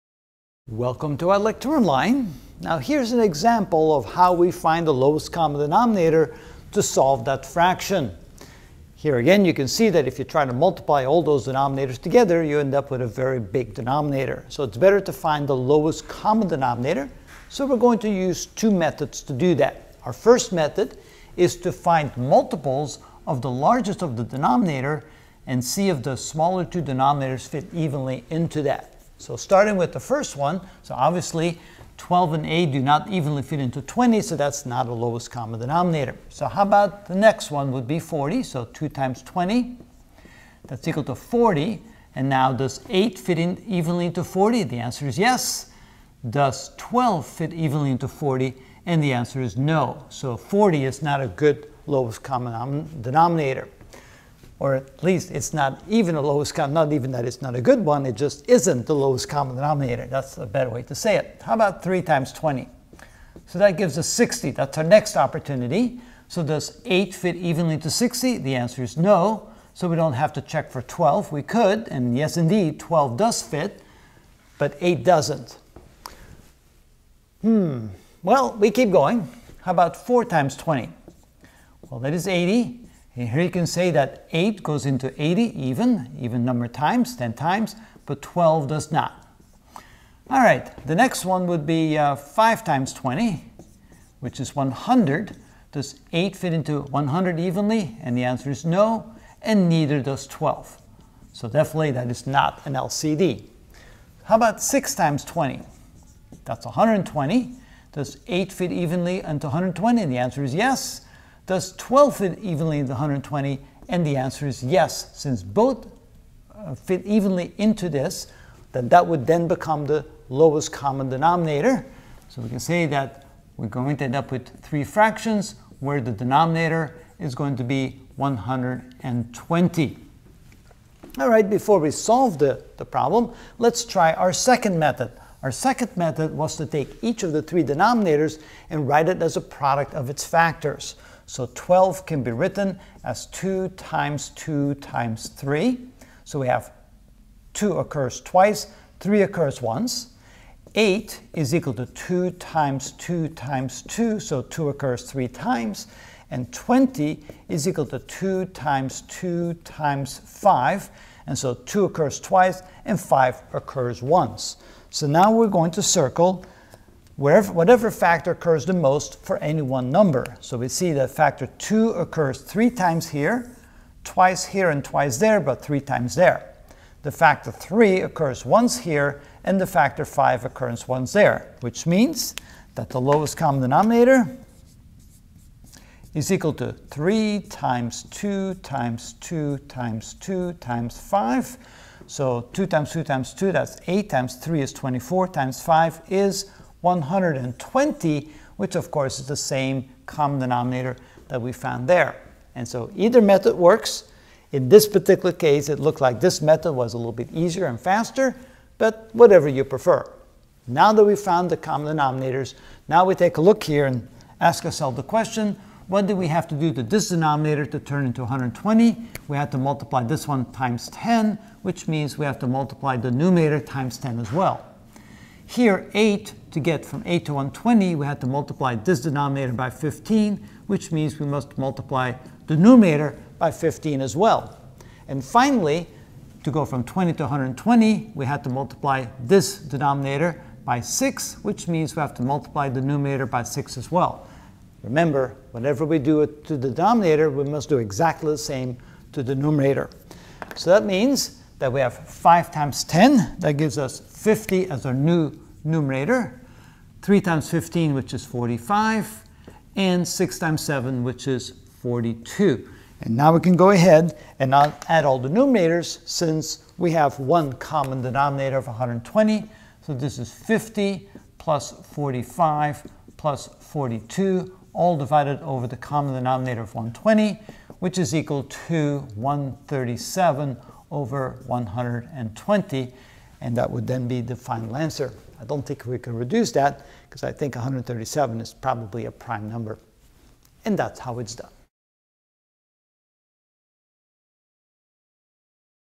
Algebra - Ch. 0: Basic Concepts (19 of 26) Adding Fractions: Method 4: Example 1
In this video I will find the lowest common denominator, LCD, to solve type 4 of adding fractions: 5/12+3/8+7/20=? Example 1